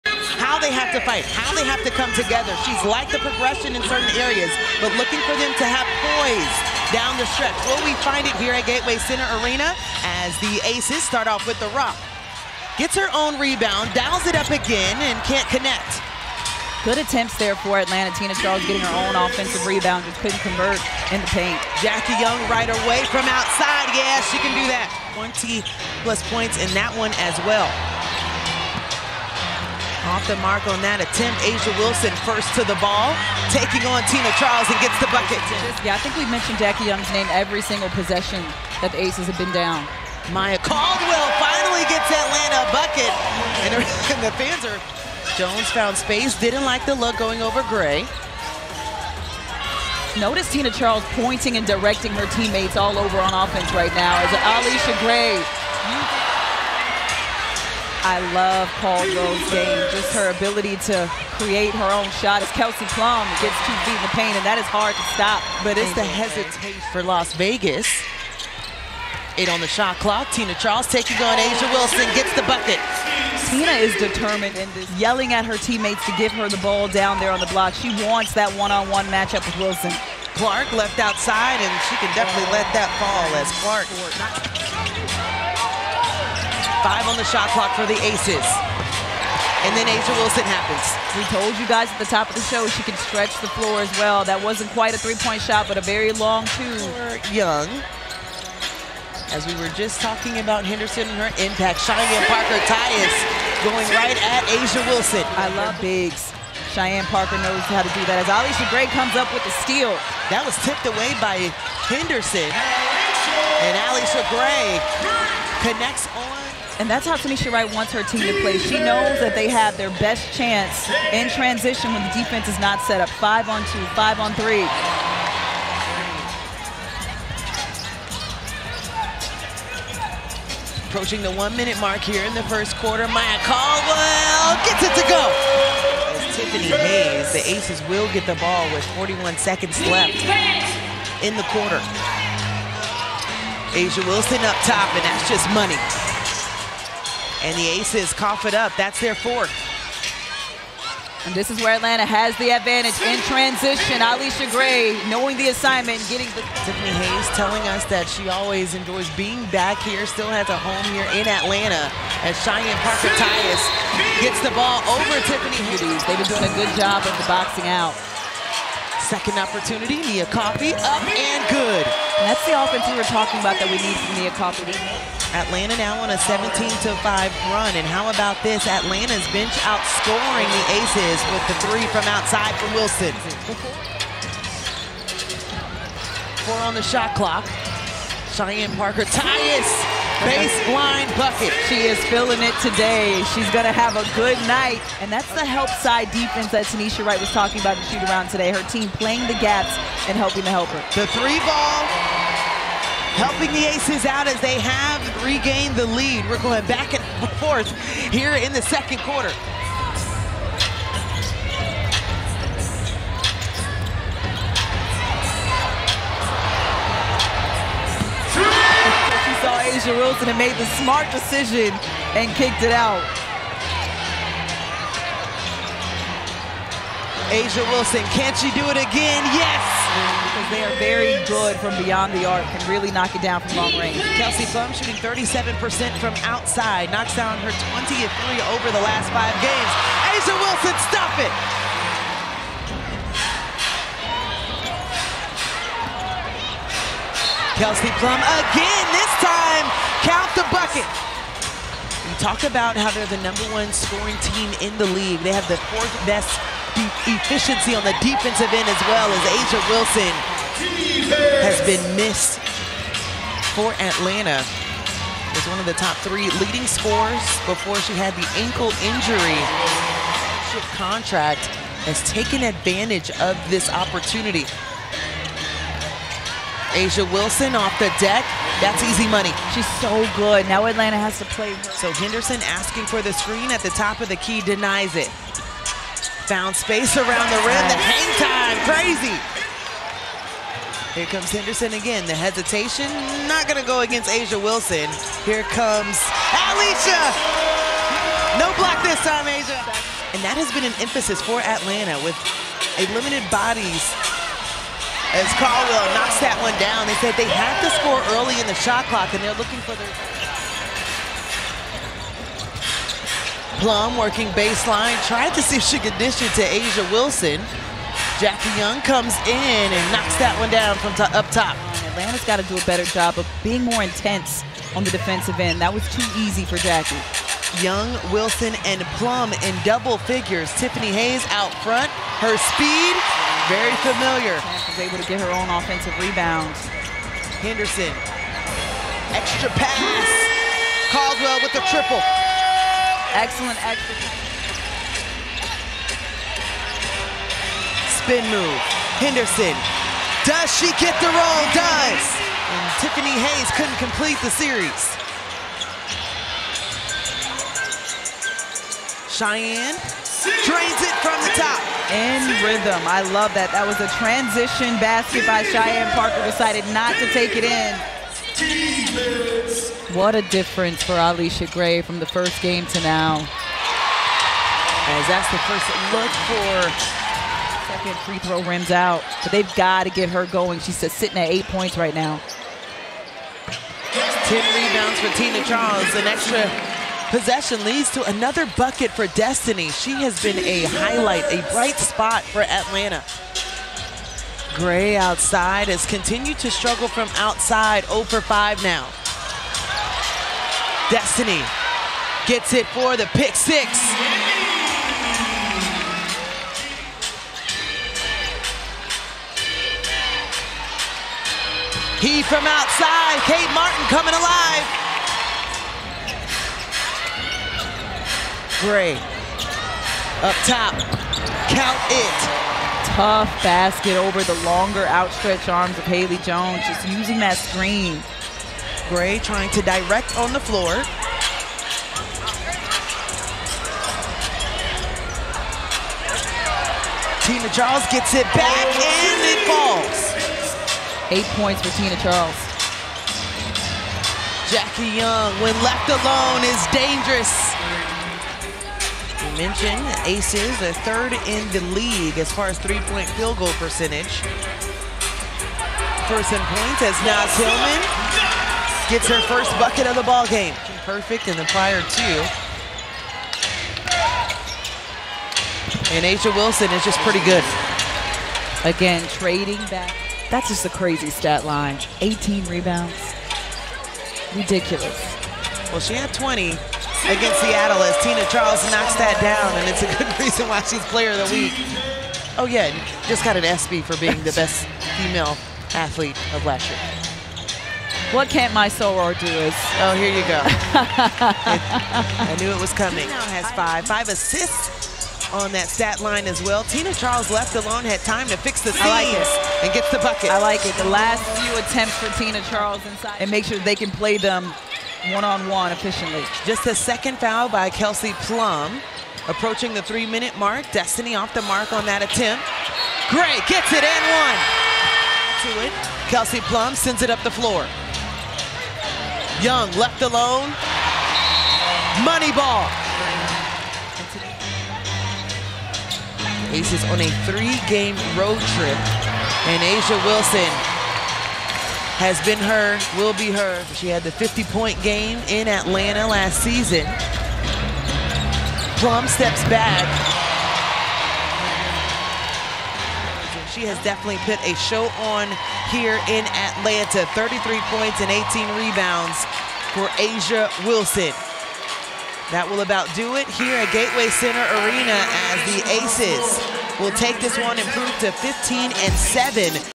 Las Vegas Aces vs. Atlanta Dream | FULL GAME HIGHLIGHTS | July 12, 2024
The Las Vegas Aces secure the 84-70 win over the Dream, powered by A'ja Wilson's MONSTER 33-point, 18-rebound performance 💥

The 28th season of the WNBA is here